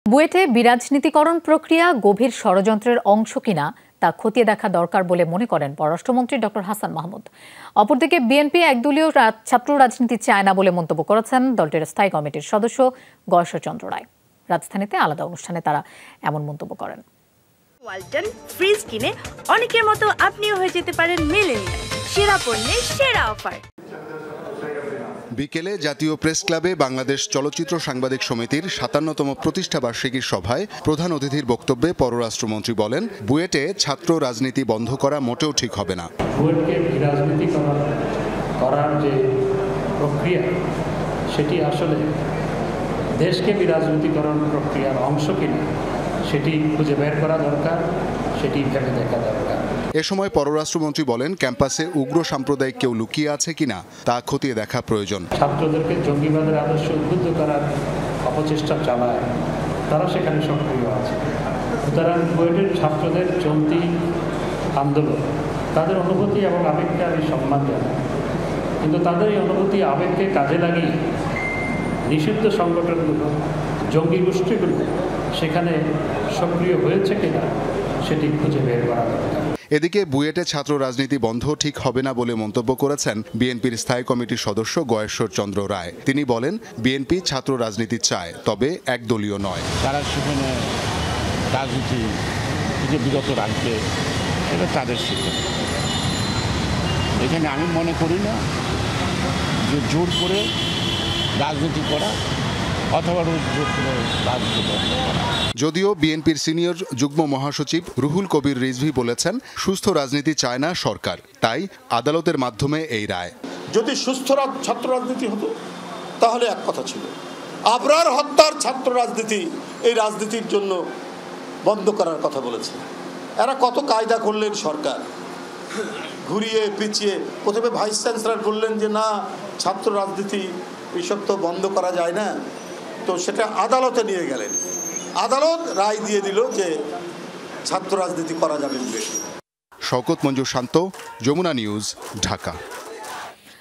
বুয়েটে একদলীয় ছাত্র রাজনীতি চায় না বিএনপি: গয়েশ্বর | Foreign Minister and BNP | Jamuna TV
বুয়েটে বিরাজনীতিকরণ প্রক্রিয়া গভীর ষড়যন্ত্রের অংশ কীনা তা খতিয়ে দেখা দরকার বলে মনে করেন পররাষ্ট্রমন্ত্রী ড. হাছান মাহমুদ। অপরদিকে বিএনপি একদলীয় ছাত্ররাজনীতি চায় না বলে মন্তব্য করেছেন দলটির স্থায়ী কমিটির সদস্য গয়েশ্বর চন্দ্র রায়। রাজধানীতে আলাদা অনুষ্ঠানে তারা এমন মন্তব্য করেন।

Address:
Bangladesh.